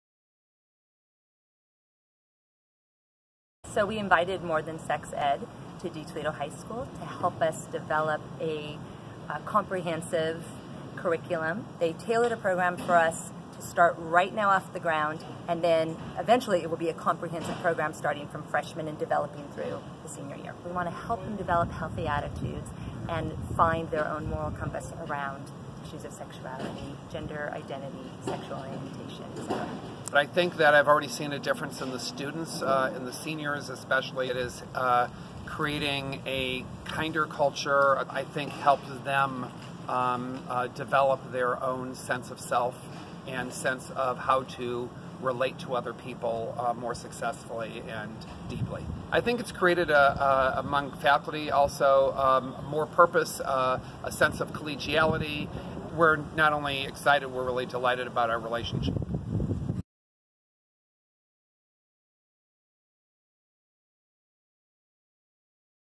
De Toledo Testimonial
Two faculty from one of More Than Sex-Ed's client schools share a little about what our program has done for their students and teachers!